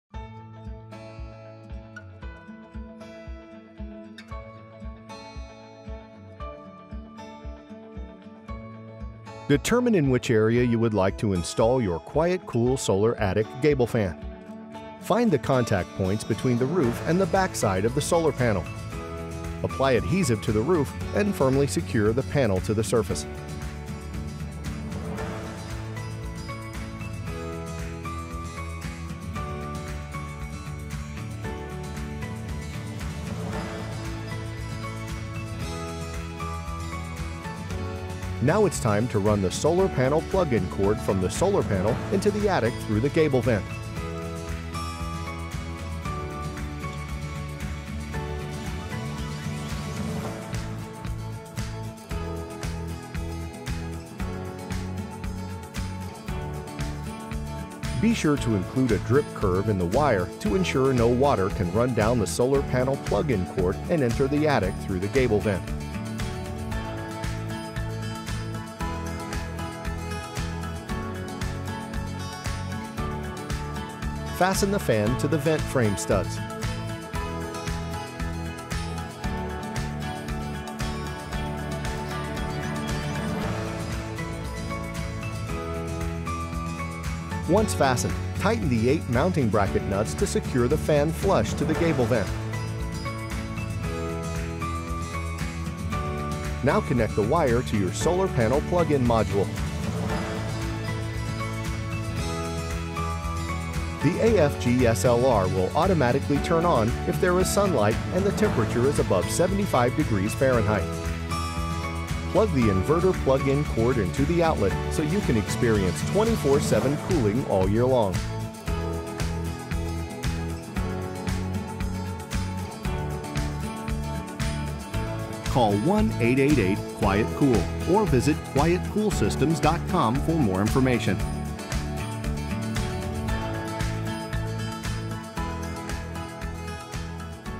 QuietCool AFG SLR Solar Attic Gable Fan Installation on Tile Roofs
QuietCool AFG SLR-40 solar attic gable fan installation on tile roofs.